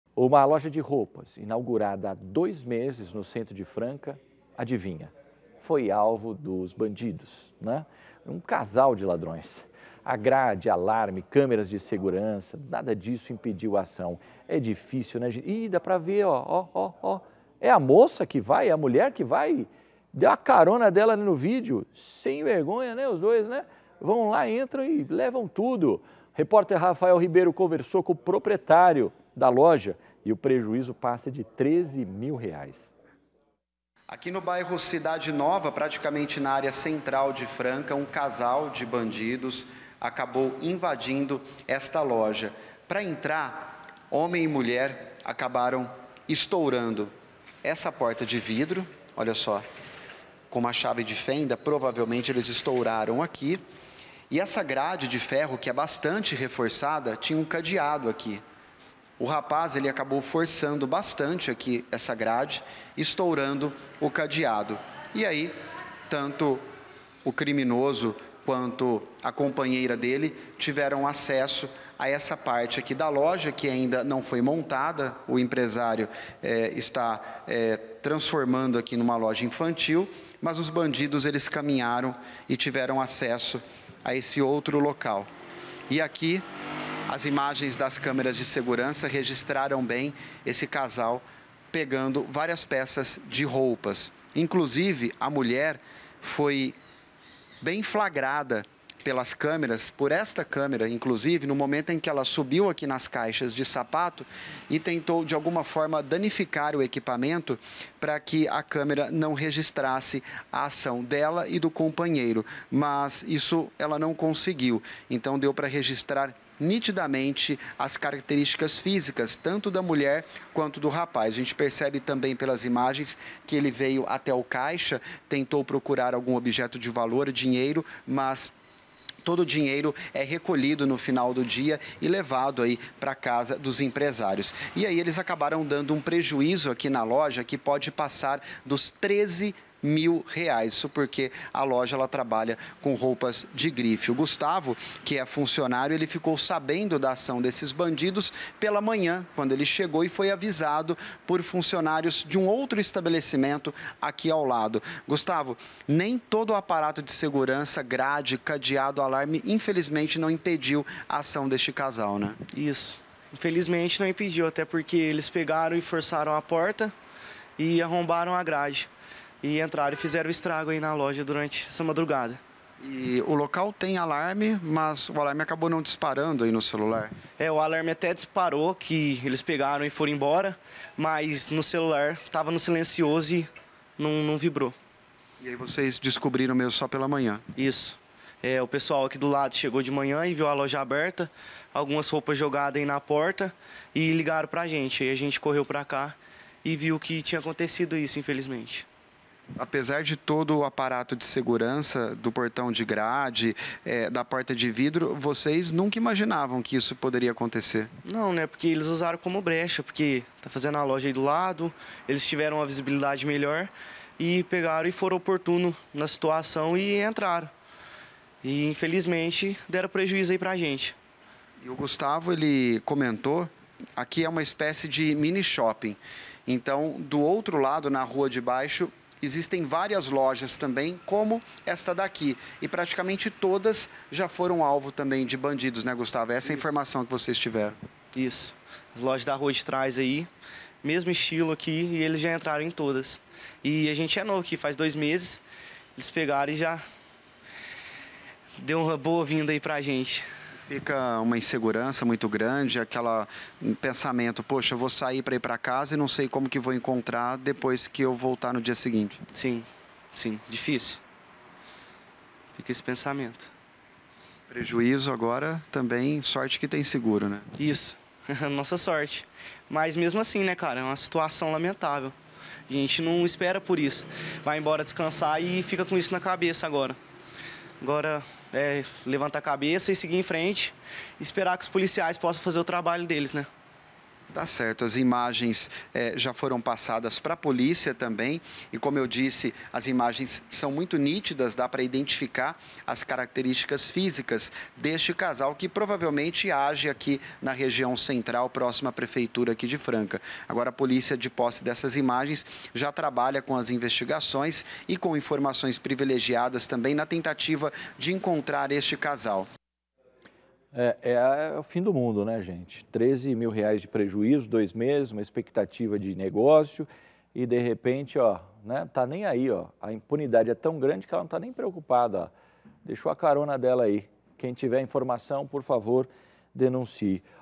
Casal invade loja, tenta quebrar câmera e faz arrastão de roupas no Centro de Franca
Uma loja de roupas inaugurada há dois meses foi alvo de um casal de ladrões na madrugada desta quarta (23), no Centro de Franca. Grade, alarme e câmeras de segurança. Nada disso impediu a ação. Segundo o proprietário da loja, o prejuízo passa dos R$ 13 mil.